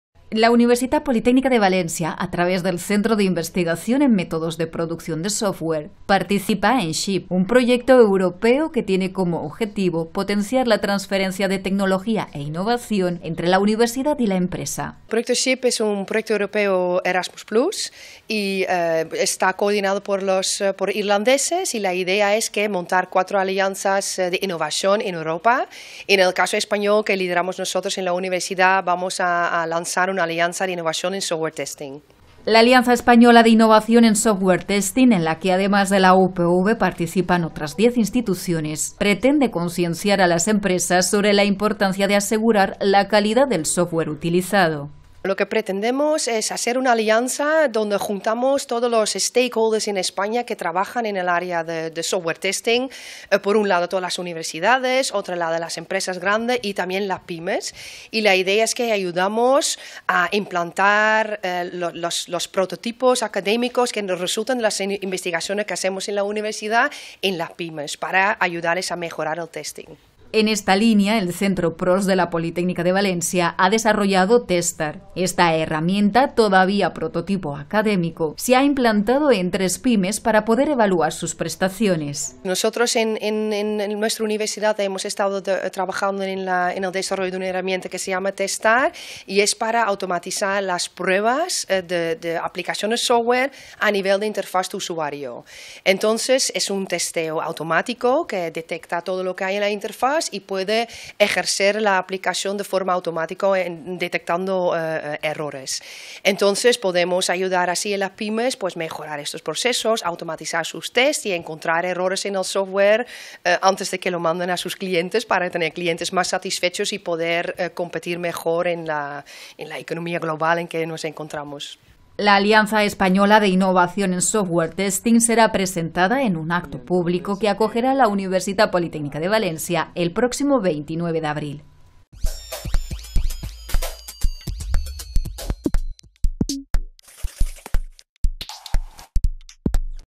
Noticia @UPVTV: Alianza Española de Innovación en Software Testing y TESTAR [02-04-2015]
La Universtitat Politècnica de València, a través del Centro de Investigación en Métodos de Producción de Software, participa en SHIP. Un proyecto europeo que tiene como objetivo potenciar la transferencia de tecnología e innovación entre la Universidad y la empresa.

La Alianza Española de Innovación en Software Testing, en la que también participan otras diez instituciones españolas, pretende concienciar a las empresas de la importancia de asegurar la calidad del software utilizado y mejorarlo.